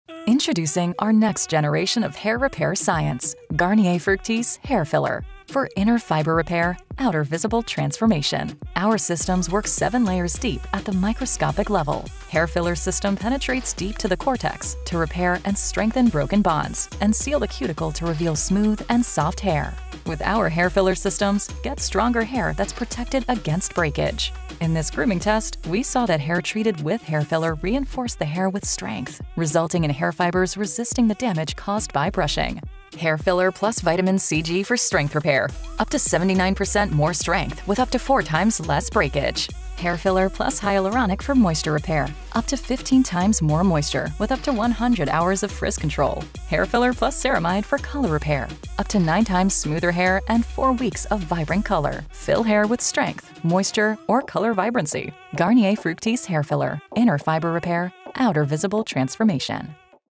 Garnier Fructis Hair Filler for Inner Repair, Outer Transformation
Introducing our next generation of hair repair science: Garnier Fructis Hair Filler for inner fiber repair*, outer visible transformation. Our systems work 7 layers deep at the microscopic level. Hair Filler system penetrates deep to the cortex to repair and strengthen broken bonds and seal the cuticle to reveal smooth and soft hair. With our Hair Filler systems, get stronger hair that’s protected against breakage. In this grooming test, we saw that hair treated with Hair Filler reinforced the hair with strength, resulting in hair fibers withstanding the breakage caused by brushing. Hair Filler + Vitamin cg for strength repair: up to 79% more strength** with up to 4x less breakage.** Hair Filler + Hyaluronic for moisture repair: up to 15x more moisture** with up to 100 hours of frizz control. Hair Filler plus Ceramide for color repair: up to 9x smoother hair** with up to 4 weeks of vibrant color. Fill hair with strength, moisture, or color vibrancy. Garnier Fructis Hair Filler: Inner fiber repair*, outer visible transformation.

*Repair and strengthen broken bonds inside the hair fiber with continuous use of system
**With use of system vs non conditioning shampoo